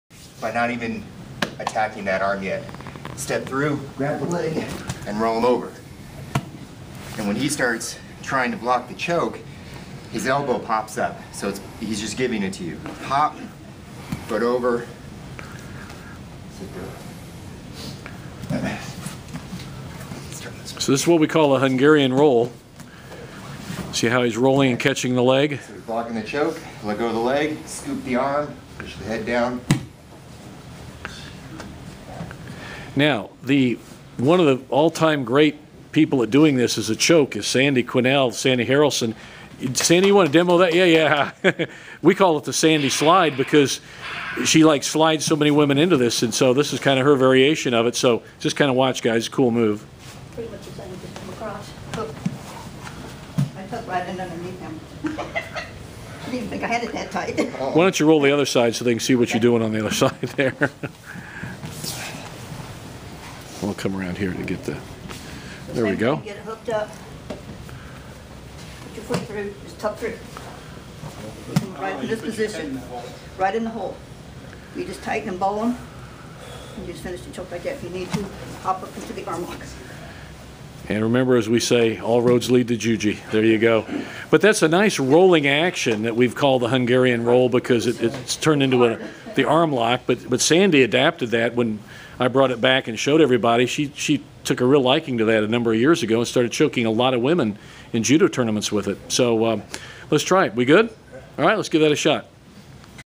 Hungarian Roll to Strangle or Armlock
Known as the Sandi Slide at Welcome Mat, this is an effective roll to an Eri Jime (Lapel Strangle) or Juji Gatame (Cross-Body Armlock). Commentary by Steve Scott. Derrick Darling and Sandi Harrelson demonstrate.